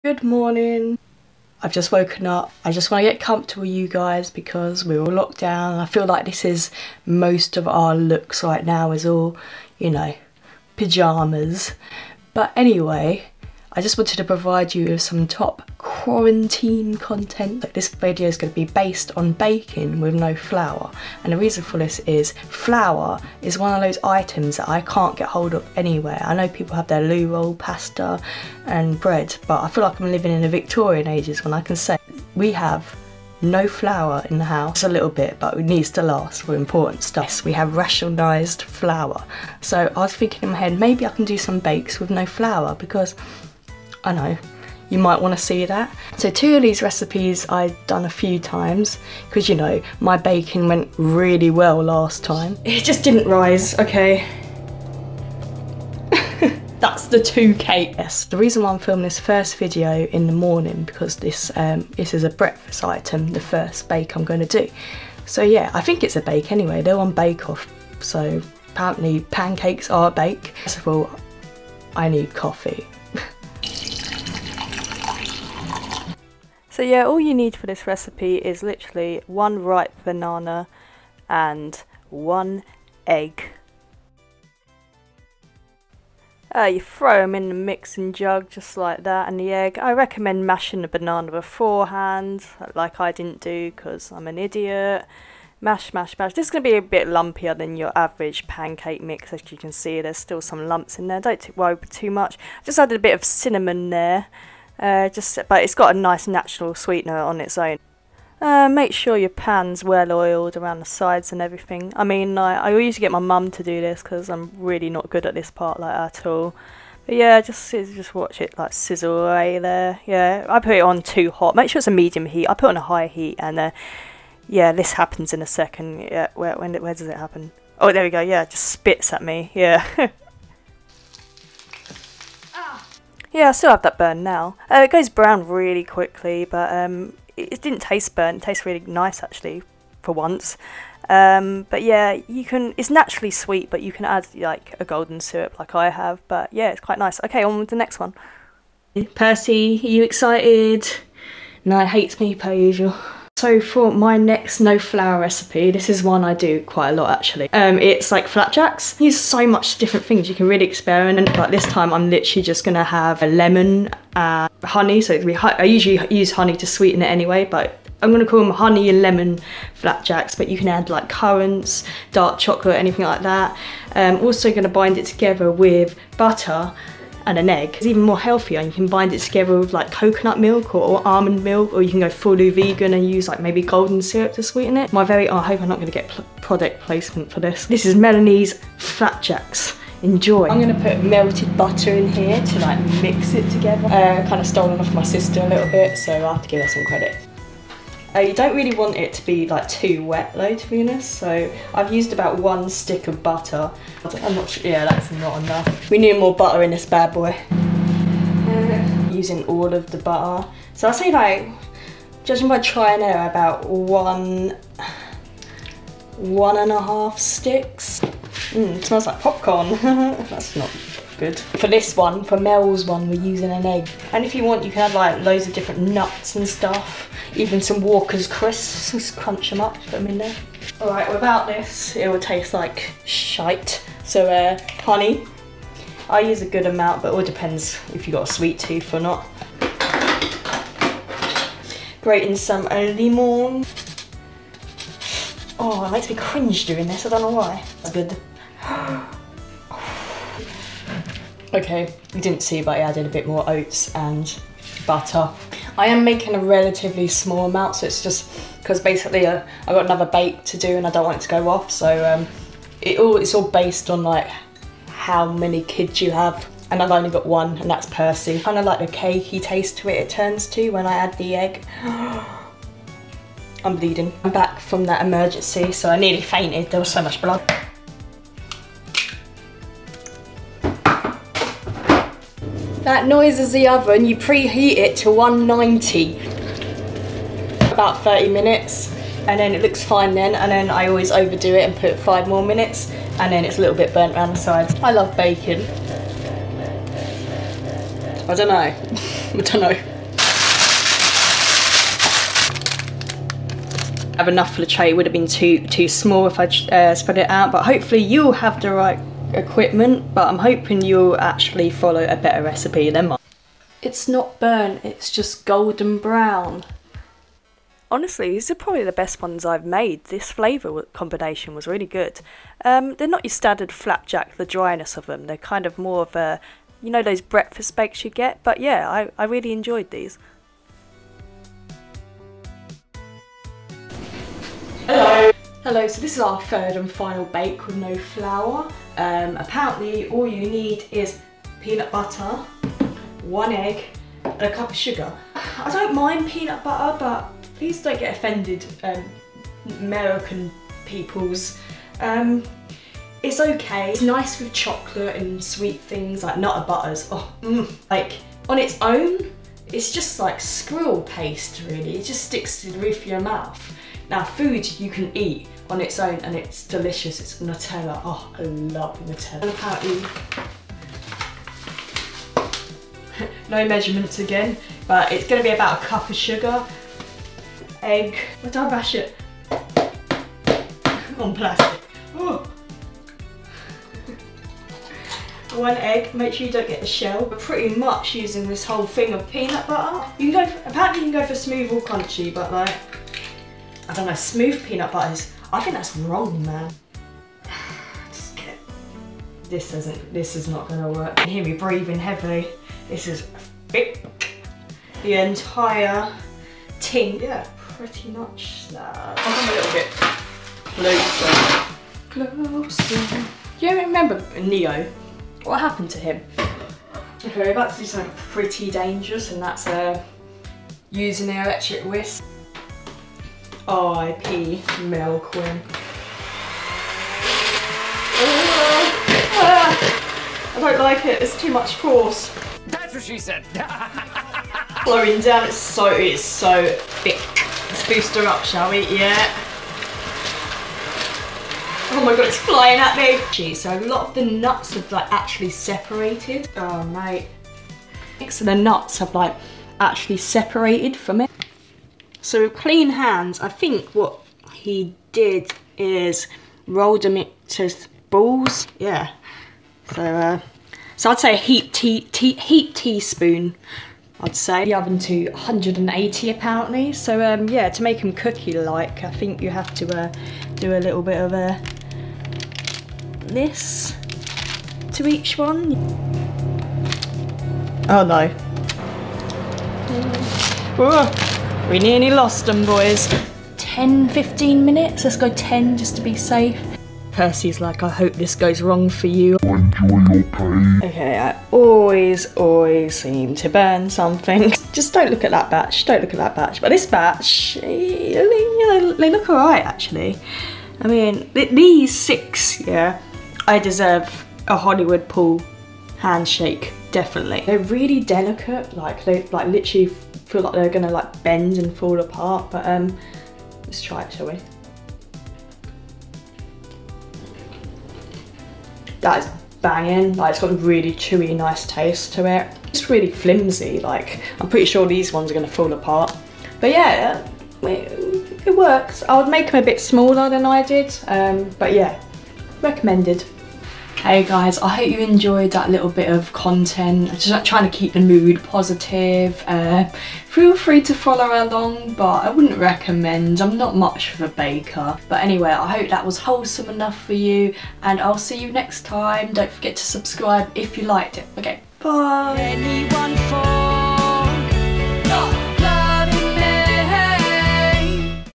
NO FLOUR BAKING!/ MEL QUINN (Isolation fun!)
My new Single on all platforms
Hope everyone is well, these were all actually very nice tbh, i forgot to say that my oven is a bit temperamental as well haha excuses! Just a bit of fun, please again if you are triggered by bakes not being perfect then don't watch ;)